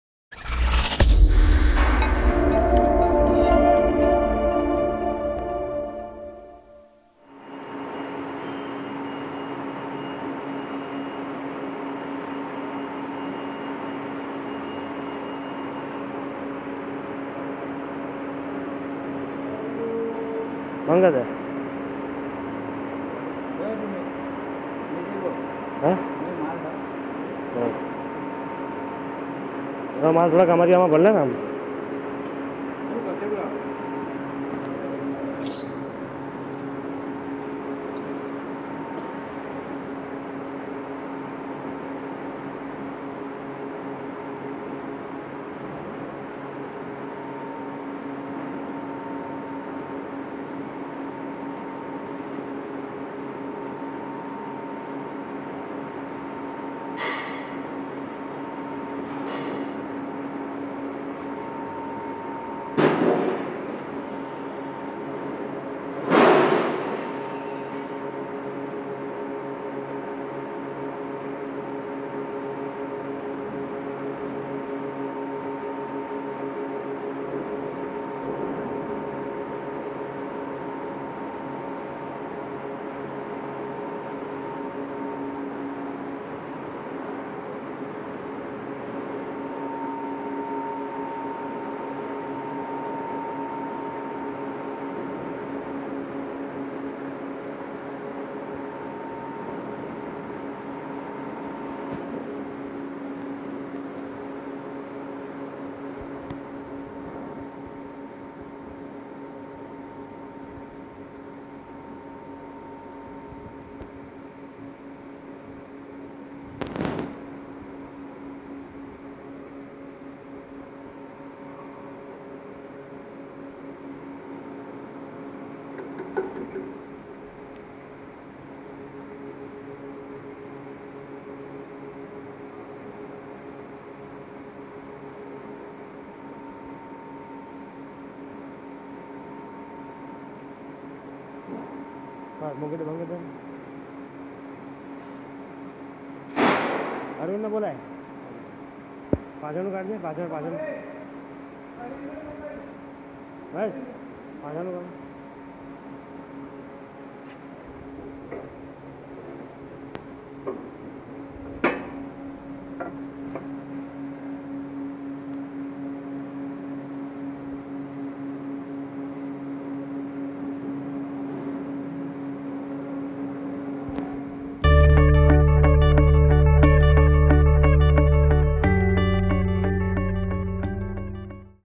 Magnetic Roller Separator - Laboratory Model
In our Roller Type Magnetic Separator - Laboratory Model, a short conveyor belt envelops the magnetic roll and tensioning ideal roll. A suitable feeder discharges materials on the belt. The ferromagnetic and paramagnetic particles adhere to the belt with the nonmagnetic particles flow freely off at the end of the conveyor.